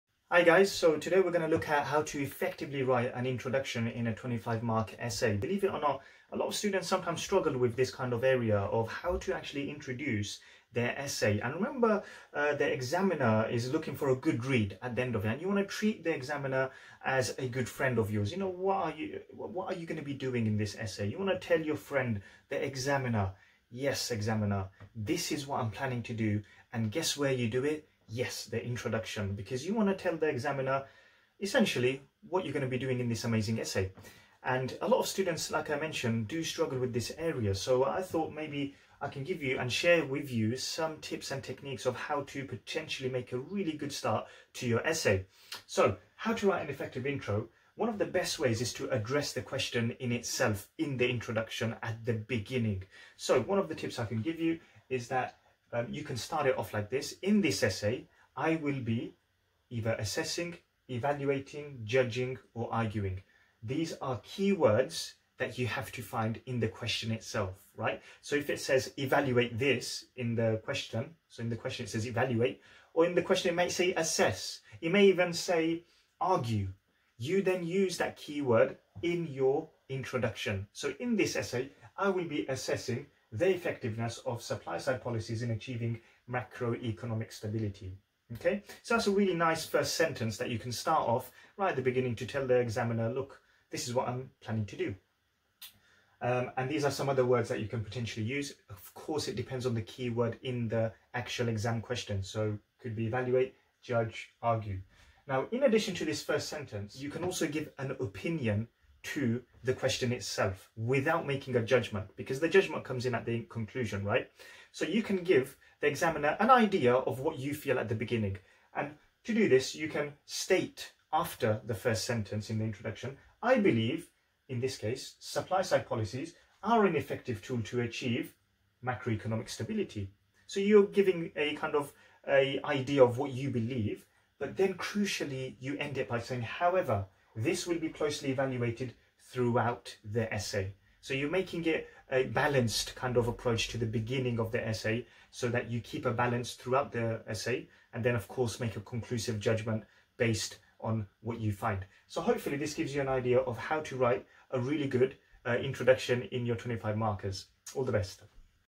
Essay Introduction in A-level Economics
Watch this video to find out super useful techniquesto help you make an awesome and concise start to your A-level Economics Essays.